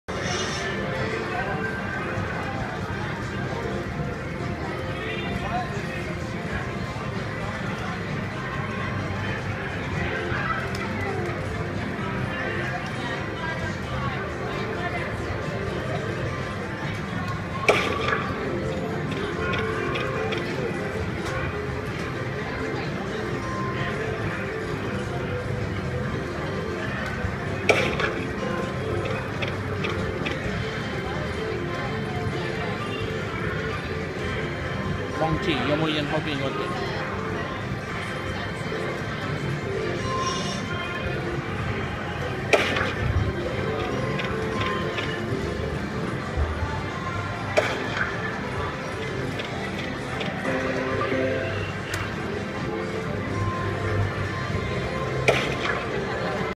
Deal or no deal-las vegas briefcase bonus #2 @ Max bet
At agua caliente casino-rancho mirage, ca